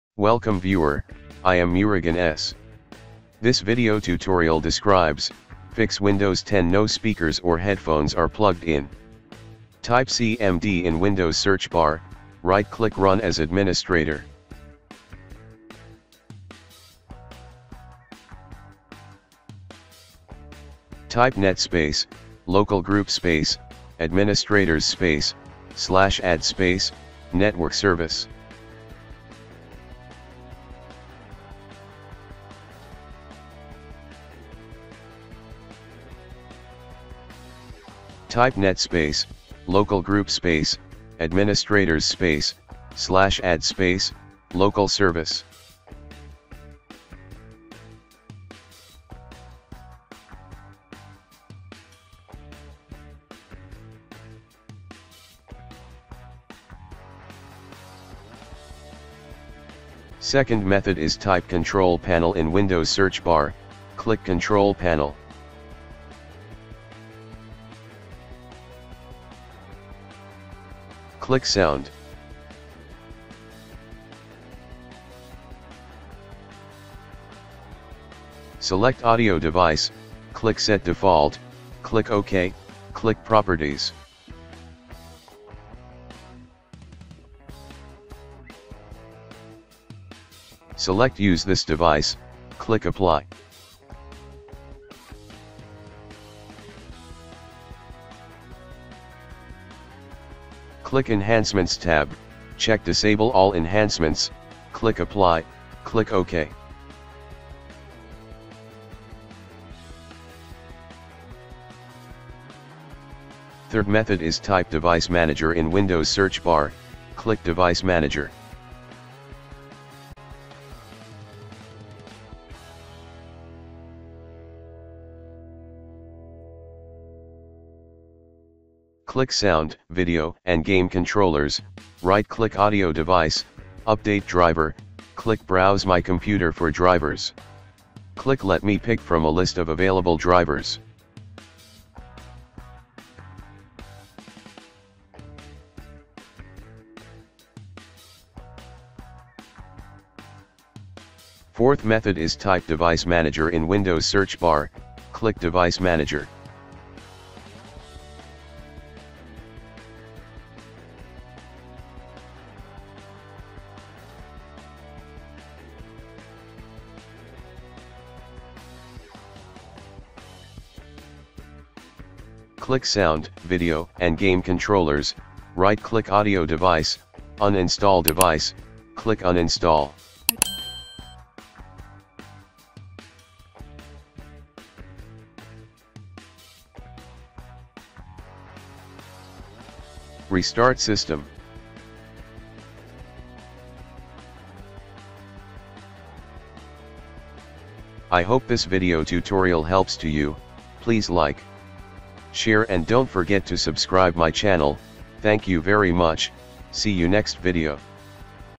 Fix Windows 10 “No Speakers Or Headphones Are Plugged In” Error (Solved)
This Tutorial Helps to Fix Windows 10 “No Speakers Or Headphones Are Plugged In” Error (Solved)

00:00 Intro
00:09 Method 1 - Add Network & Local Service
01:00 Method 2 - Change Sound Settings
01:53 Method 3 - Update Audio Device (Browse from My Computer)
02:31 Method 4 - Uninstall Audio Device
03:17 Closing